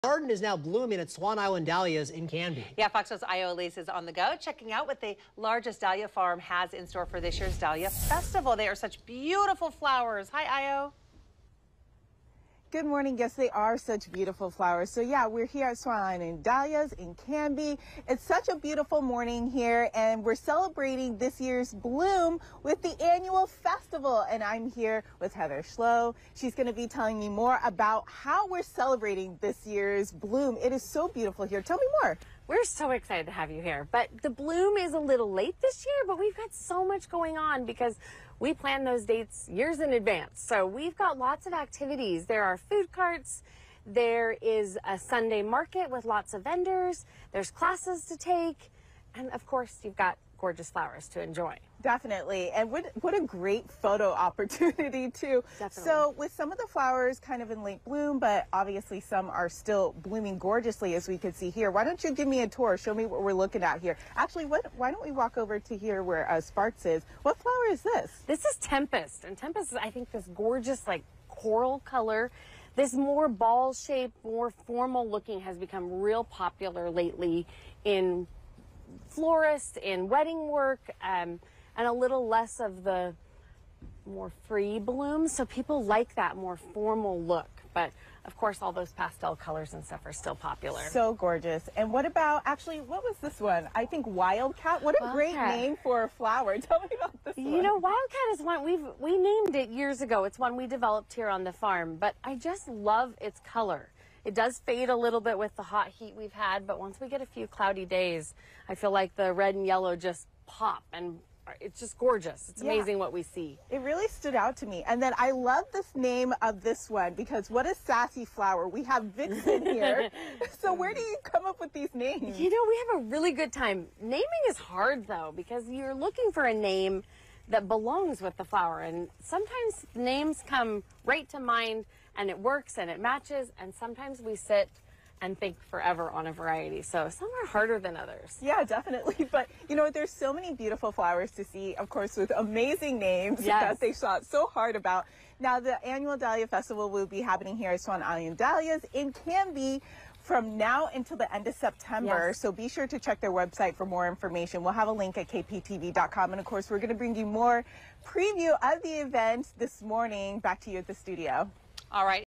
On the Go at Swan Island Dahlia Festival in Canby
The flowers at Swan Island Dahlia’s in Canby are popping up in all colors, shapes and sizes.